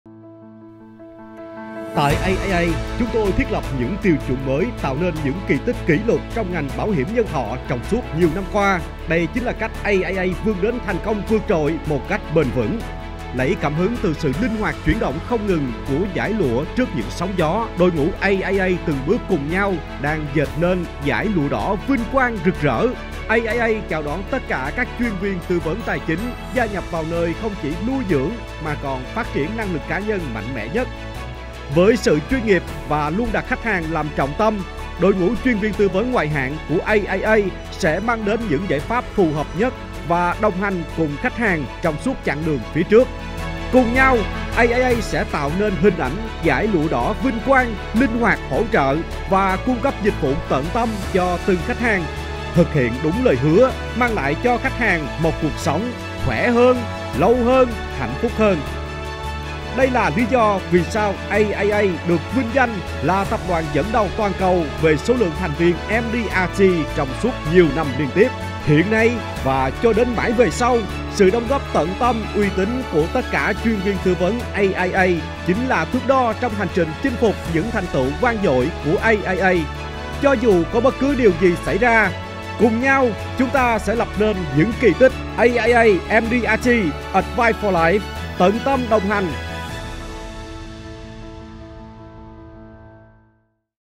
THƯỚC PHIM GIỚI THIỆU CVTV MDRT CỦA AIA| AIA MDRT. ADVICE FOR LIFE
Hình ảnh "Dải lụa đỏ vinh quang - Red Fabric" luôn không ngừng chuyển động, cùng với màu đỏ tượng trưng cho sự vinh quang trong sự nghiệp khi đạt danh hiệu MDRT. Hình ảnh mới này nhằm mục đích tôn vinh các giá trị của các thành viên MDRT Tập Đoàn AIA.